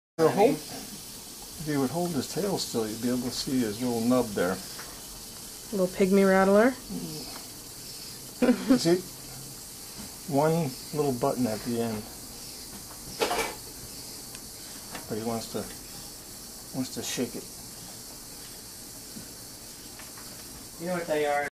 Pygmy Rattlesnake
Our friend Mike holding one of our Pygmy rattlesnakes.